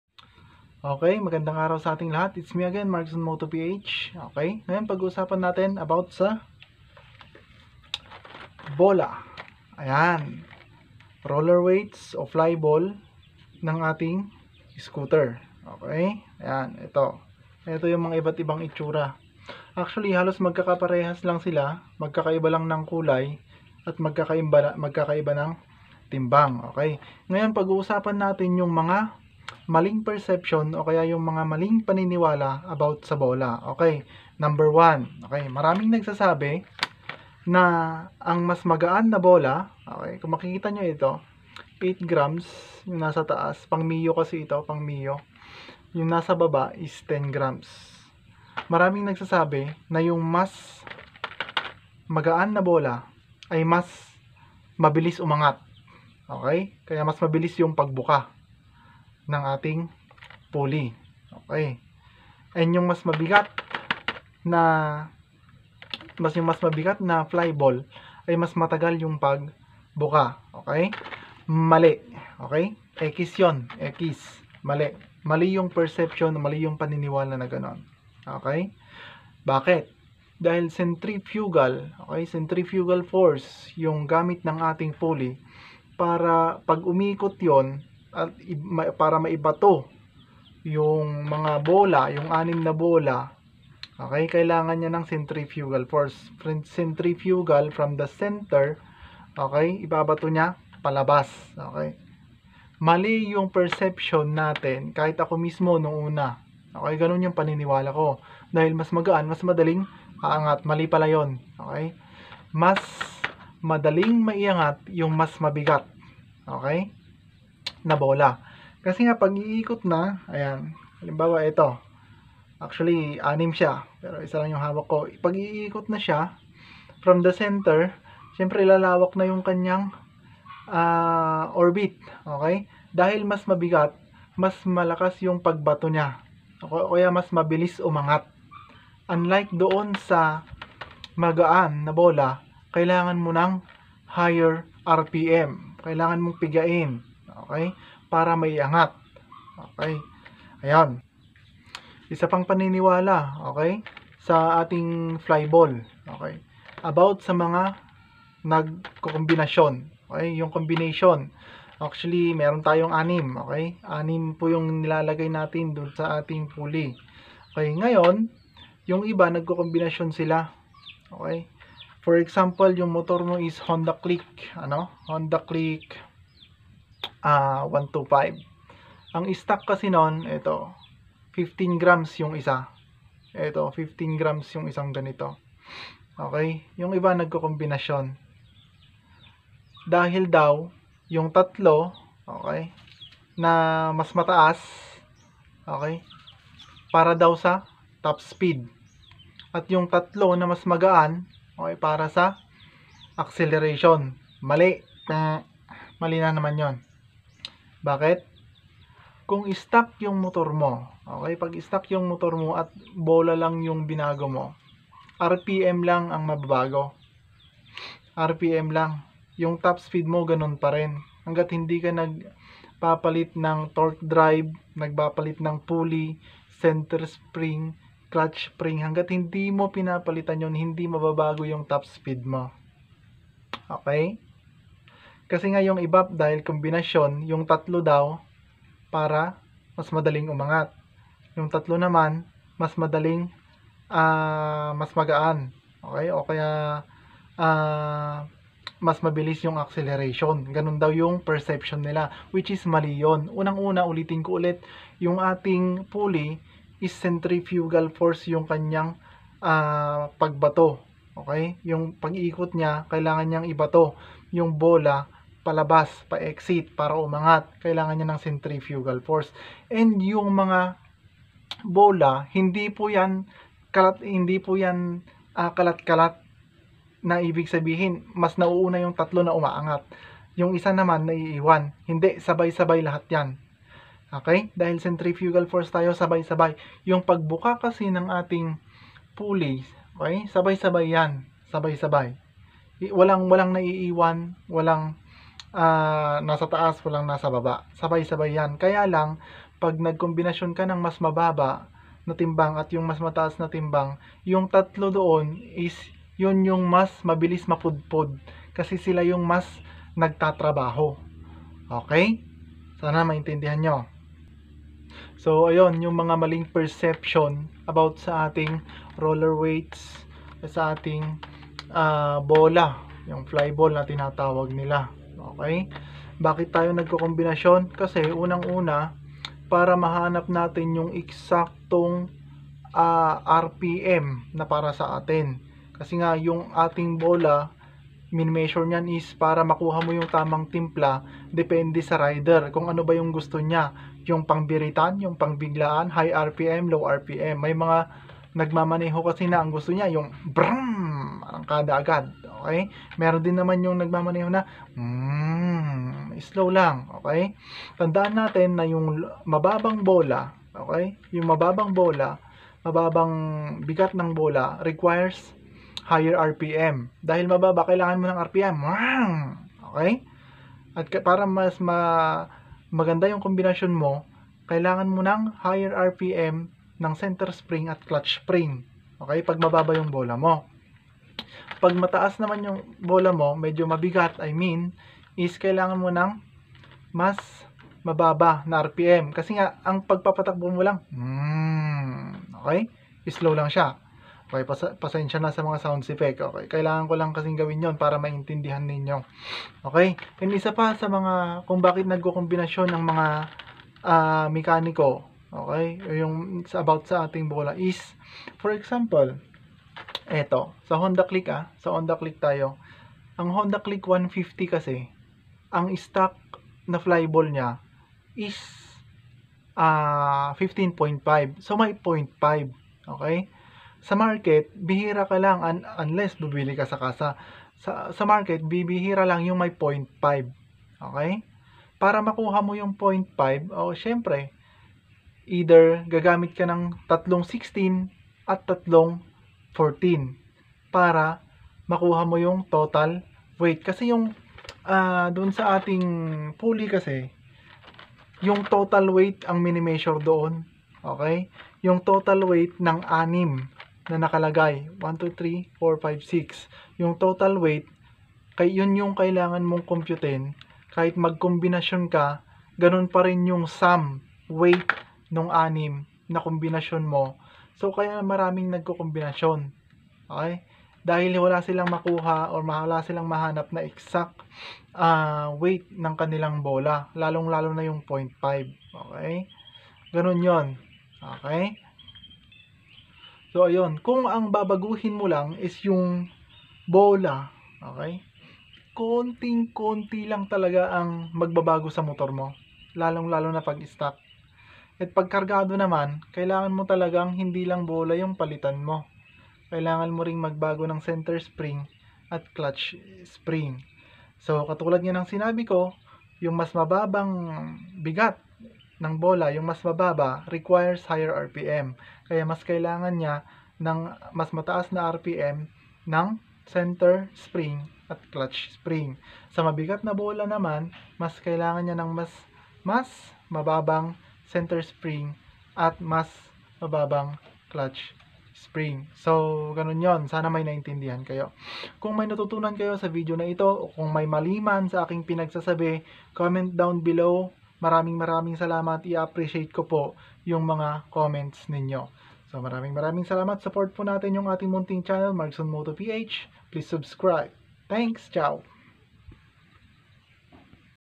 Mga maling paniniwala sa Flyball/Roller Weights sa CVT Scooter
Sa video na ito share ko mga natutunan ko about sa CVT ng Scooter. Gusto ko lang ibahagi mga natutunan ko sa pag gamit ko ng scooter.

MarcsonMoto PH